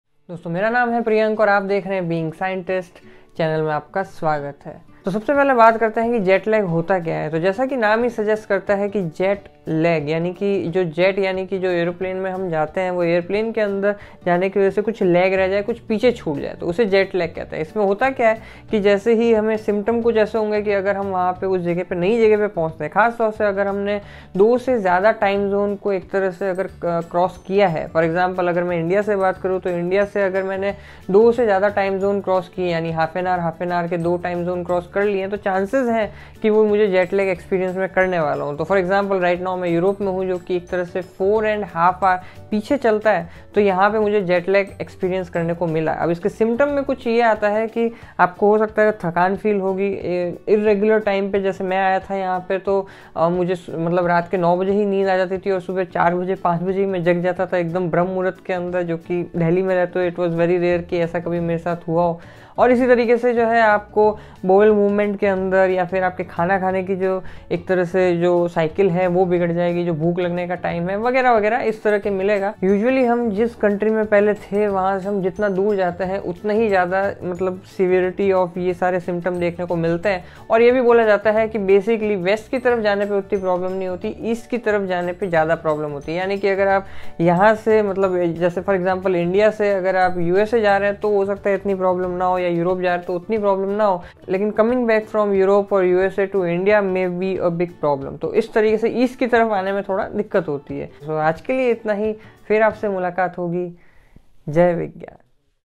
What is Jet Lag? | Hindi | Dr. Priyank Singhvi
Jet lag is when your normal sleep pattern is disturbed after a long flight. It usually improves within a few days as your body adjusts to the new time zone.

जेट लैग तब होता है जब लंबी उड़ान के बाद आपका सामान्य नींद पैटर्न गड़बड़ा जाता है। यह आमतौर पर कुछ दिनों के भीतर सुधर जाता है क्योंकि आपका शरीर नए समय क्षेत्र में समायोजित हो जाता है।

Videomaker, editor and narrator: Dr. Priyank Singhvi